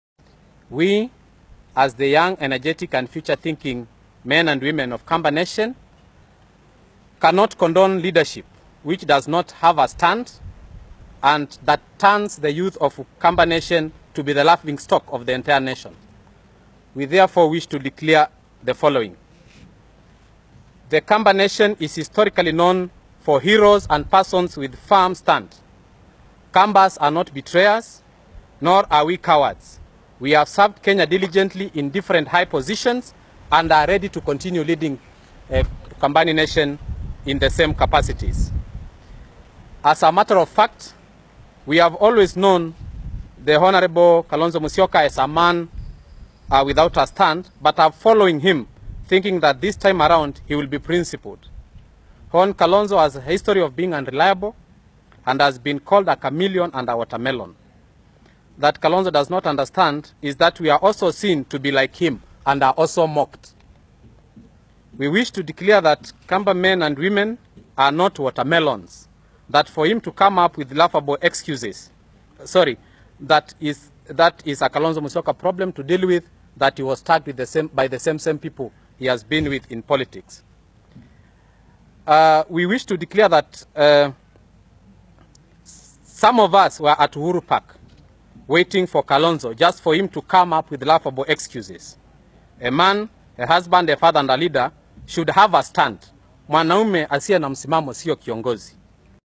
Kamba Youth Angry Reaction to Kalonzo Musyoka's Absence at NASA leader swearing in
Young kamba leaders are not happy with their leaders disappearance during the people's President inauguration